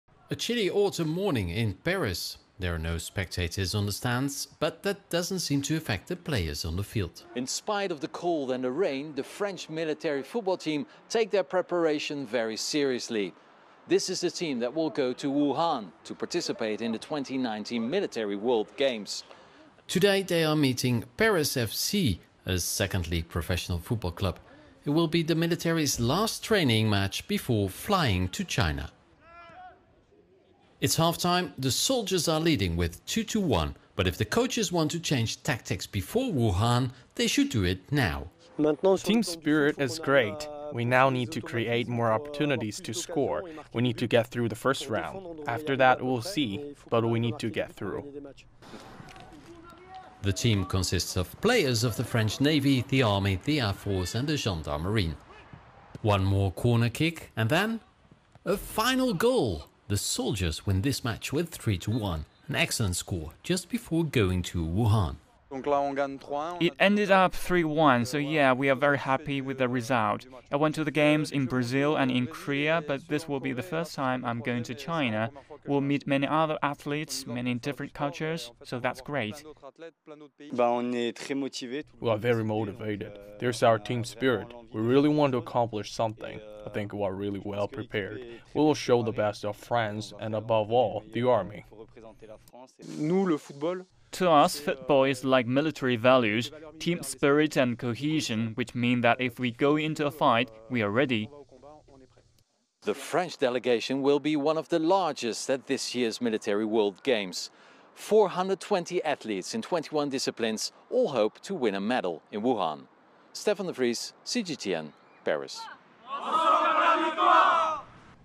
French football team making final preparations
The French military football team are making their final preparations before they fly to Wuhan for the games. They say they've left no stone unturned in their preparations.